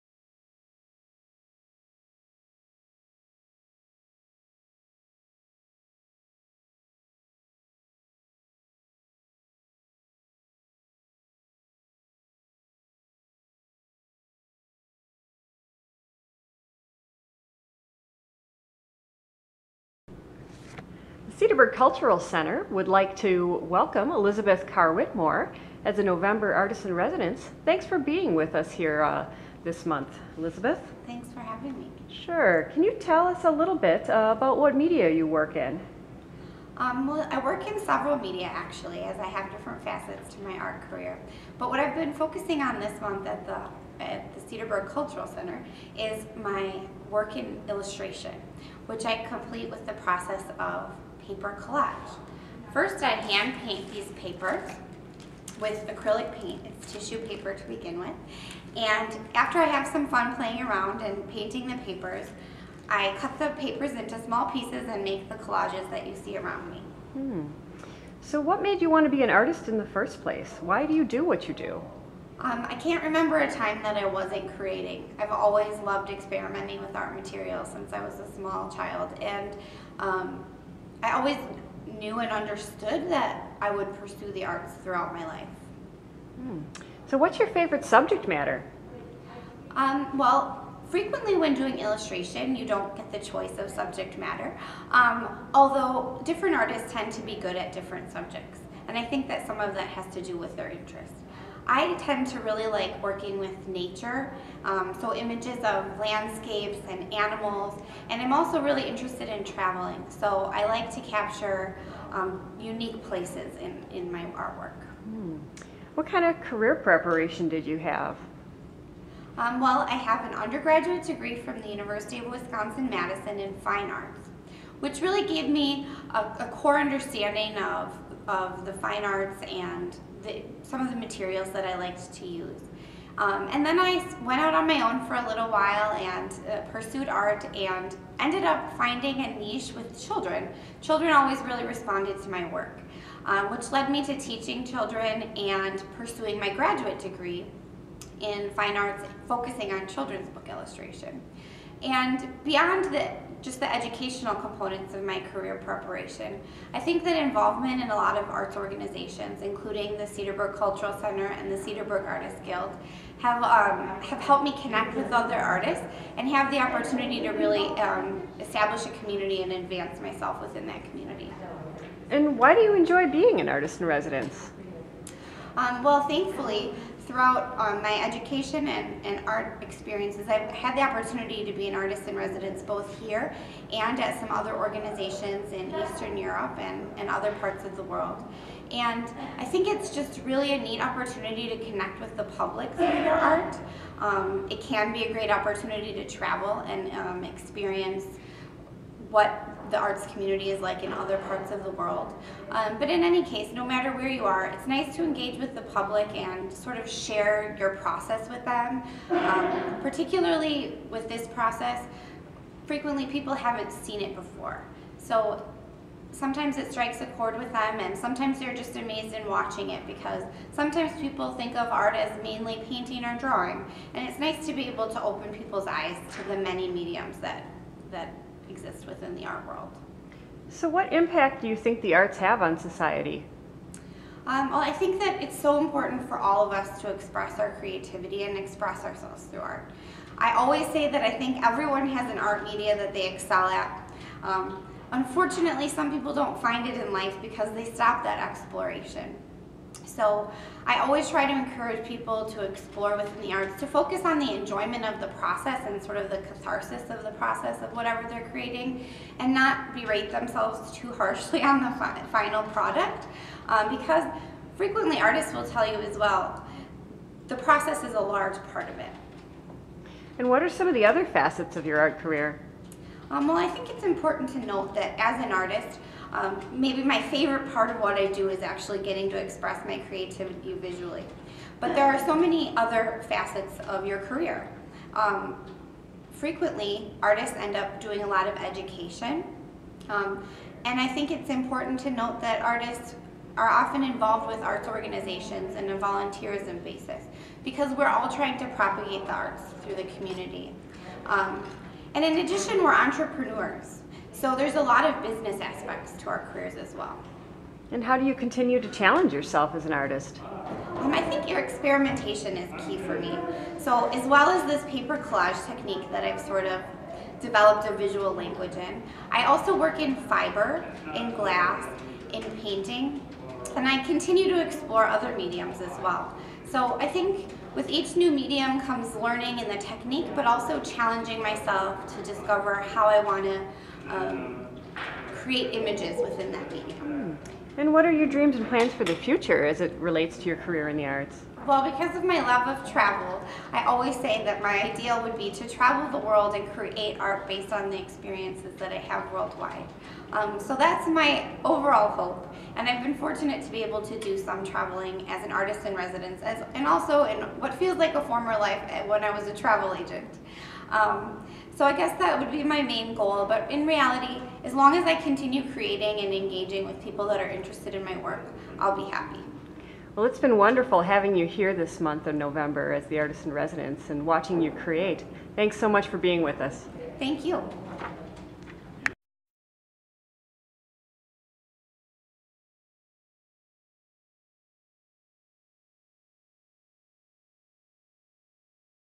Elizabeth Carr Whitmore: Artist in Residence at the Cedarburg Cultural Center
After completing her undergraduate degree from the University of Wisconsin-Madison, West Bend artist and illustrator Elizabeth Carr Whitmore undertook a year of national service with Americorps, managed a travel agency, developed art education programs at two community centers, and directed an art summer camp.  She went on to receive her Master of Fine Arts degree in illustration at the Academy of Art University.  Elizabeth dreams of traveling the world, experiencing diverse landscapes and cultures and recording these experiences through art, illustration and design.